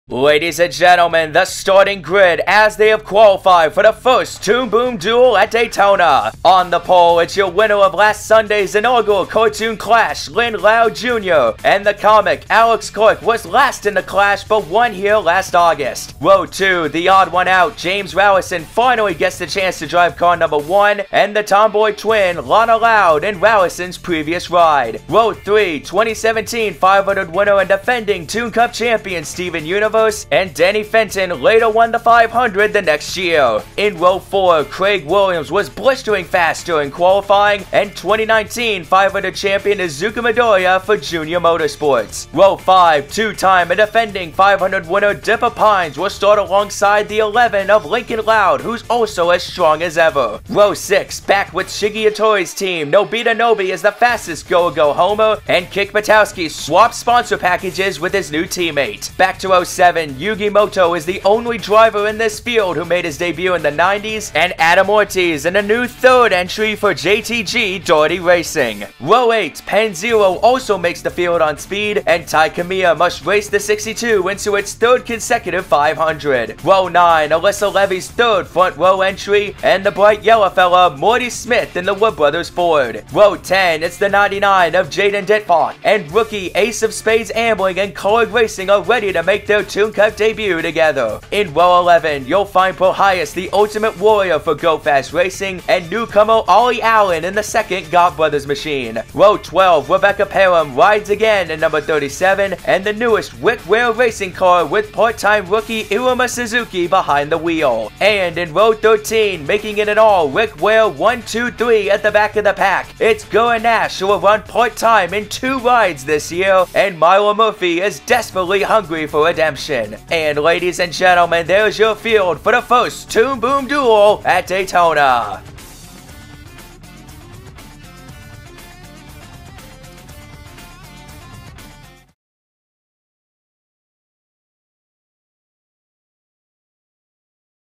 (NASCAR TOON CUP FANFIC) 2021 1st Toon Boom Duel Starting Grid
Race one of two to see who makes the starting field for the 33rd annual Daytona 500!

My fanfiction series was inspired by Michael Nebbia a.k.a. BurstFlame24 on DeviantArt. Check him out here: